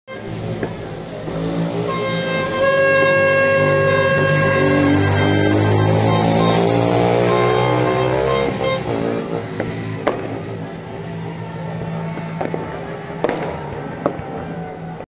2024 New Year's in Baguio - Energy dying down
Fewer fireworks and softer horns.

Original title:
VID_20240101_001113.mp4